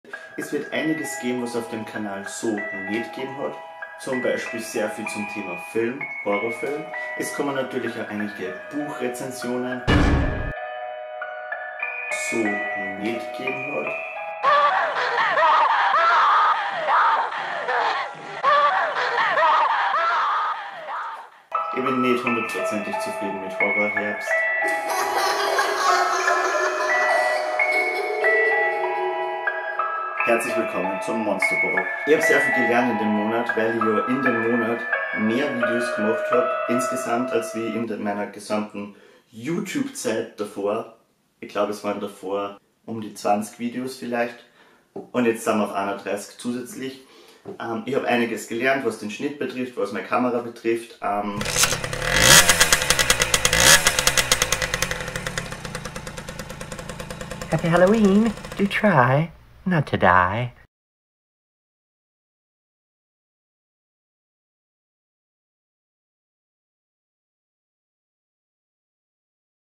ES GEHT LOS! - Trailer / Monsterball 01 / LostBooks
Willkommen, in der Infobox des Monsterballs!

Monsterball bedeutet: 31 Videos im Oktober. 4 verschiedene Themenwochen. 6 Youtuber. Und jede Menge Buch-, Film- und Horrortipps!

Seht es als euren eigenen Halloween-Adventkalender und feiert mit uns die schönste und grusligste Jahreszeit.

Heute starten wir mit einem kleinen Trailer, ich hoffe ihr seit genauso gehyped wie ich und morgen geht’s auch schon weiter.